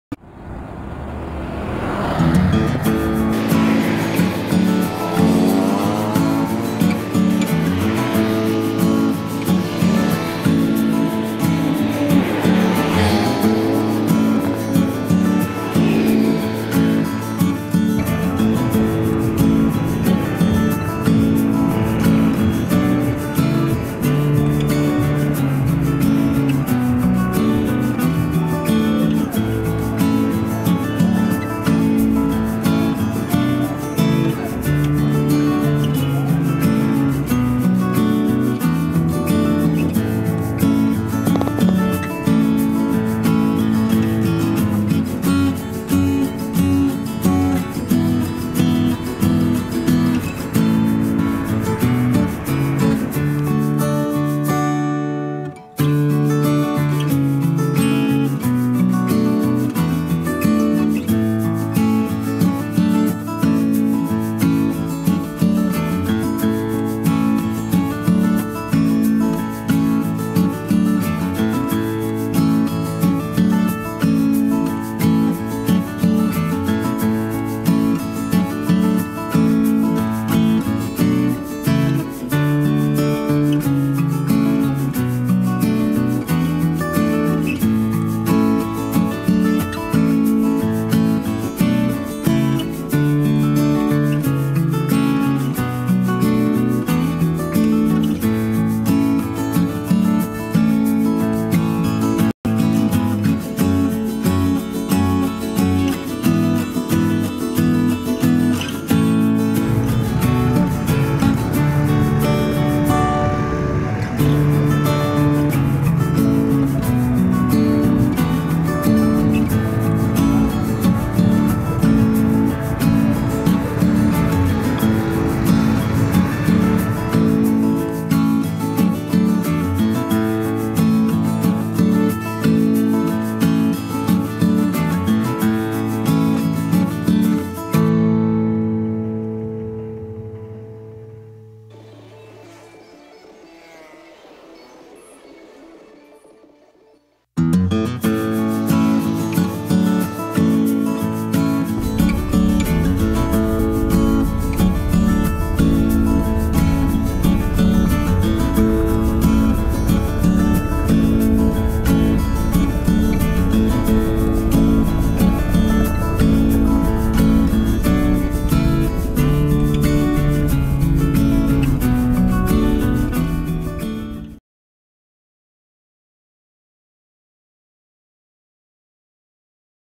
Kota Ambon 2020| Tugu Leimena
Berada di Tugu Leimena bundaran Poka Ambon. Terbaru Januari 2020
Ikuti Kota Ambon Manise. Kota ambon 2020